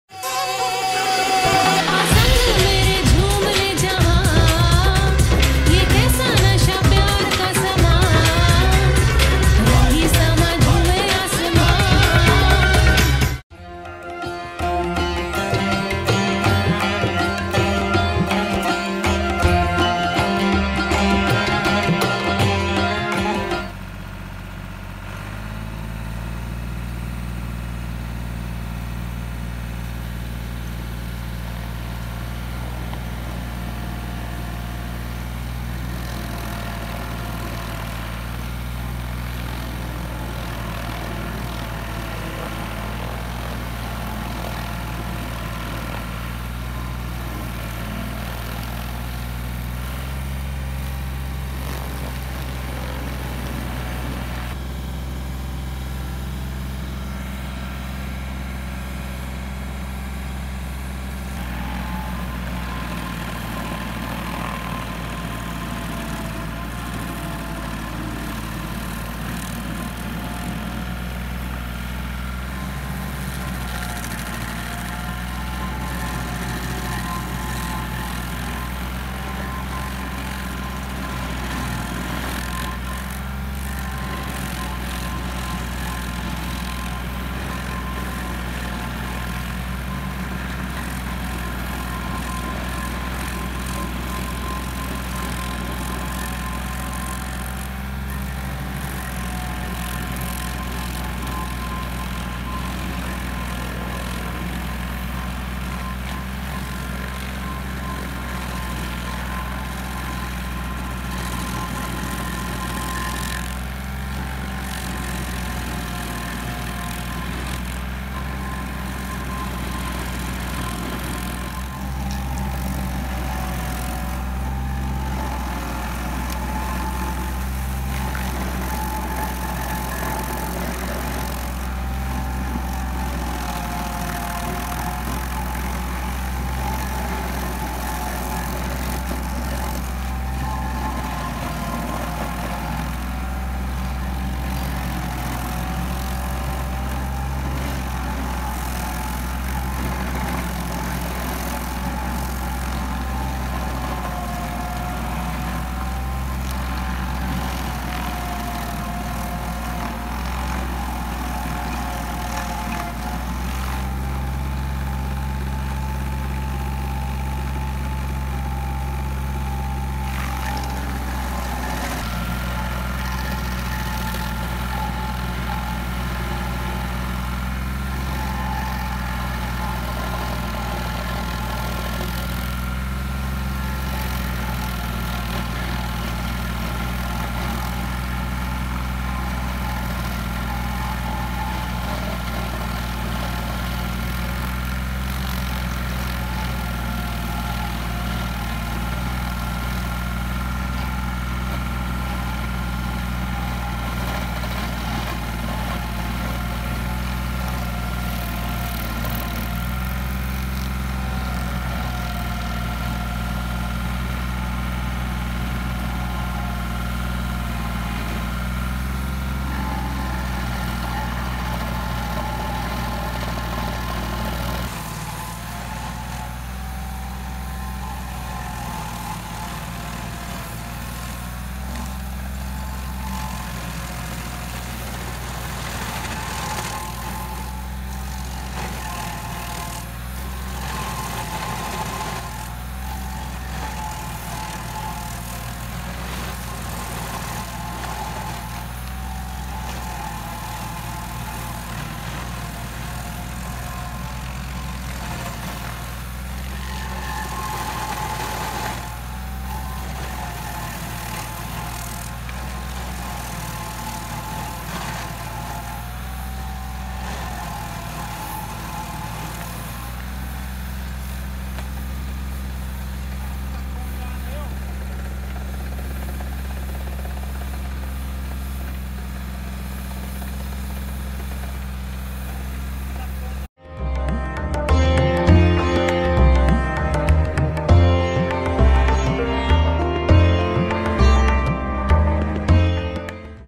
Best Way to remove Tree Stump ? Meet The Beast!
We are busy with our new home, several projects. This video captures tree removal process, grinding out stumps, fastest way to remove large stumps! This machine is a beast, had fun filming it and hope you like it too.